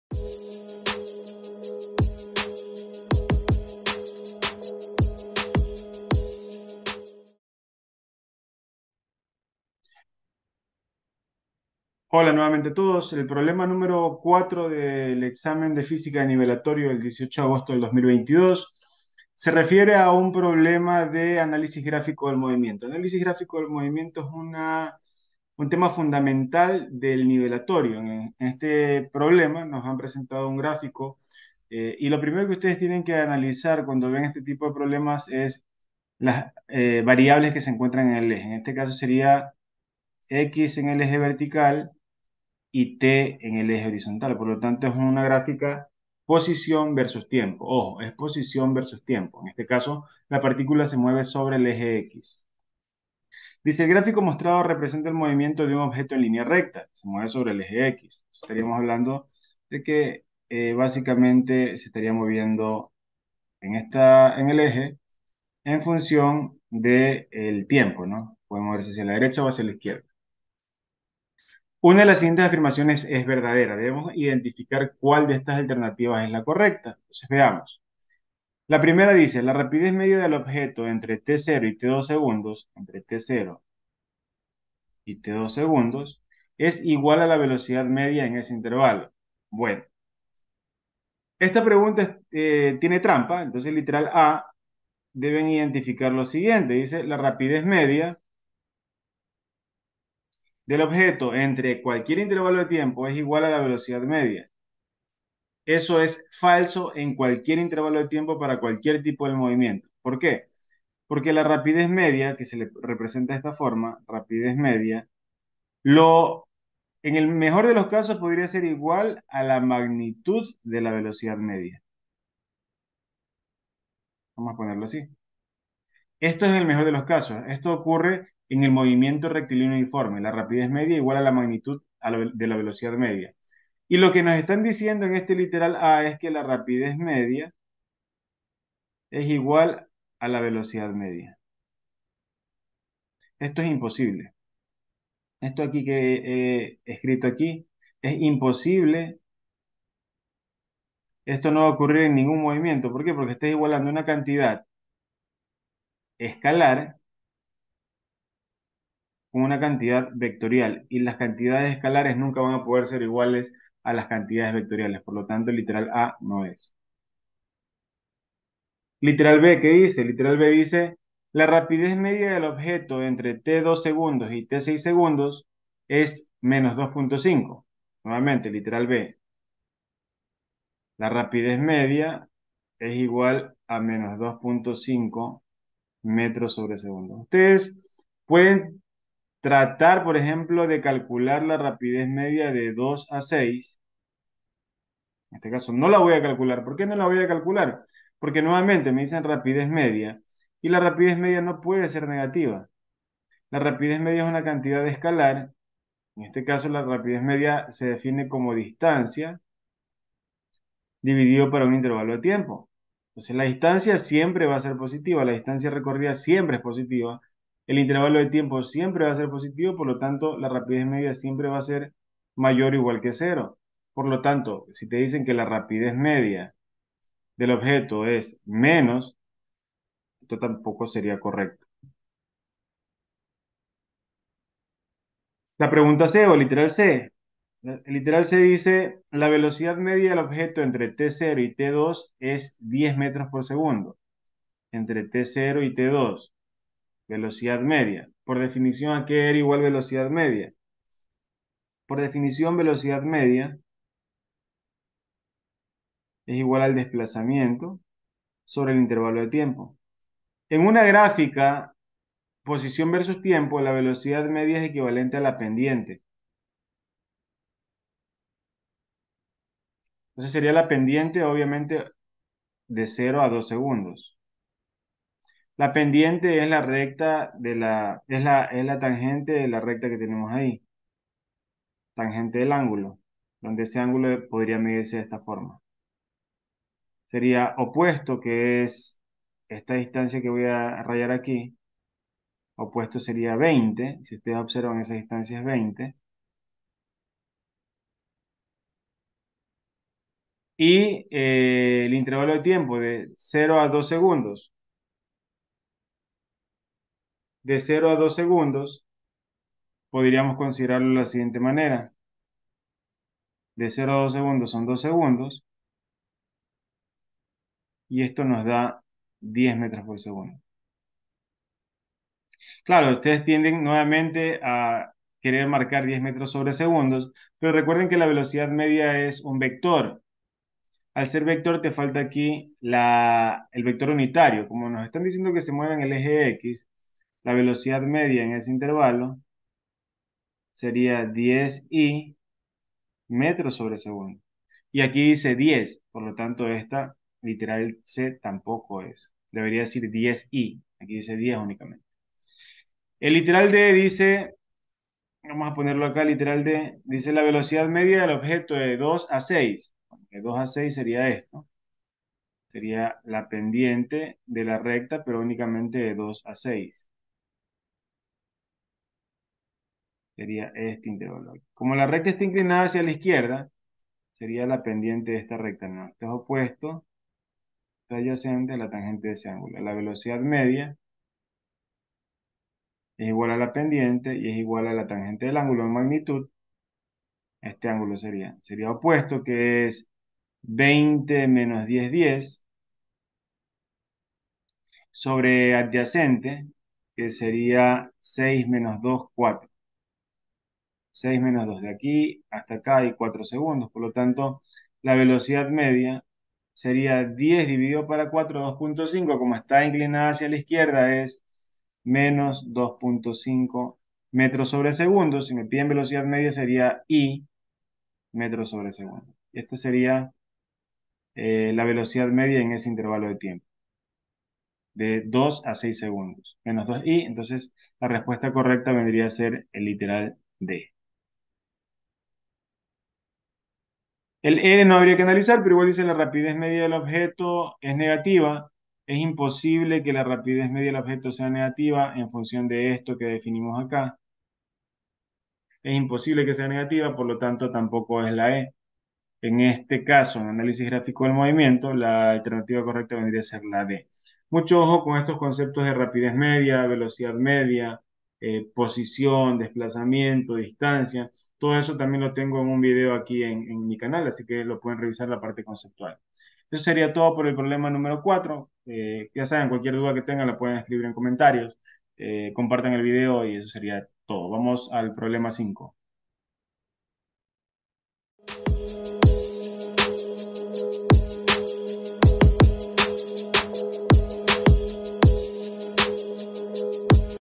Problema 4 - Examen de Nivelatorio - Física - Análisis gráfico del movimiento - MRU - ESPOL
Resolución de un problema de Análisis gráfico del movimiento. Cinemática.
Velocidad media y rapidez media.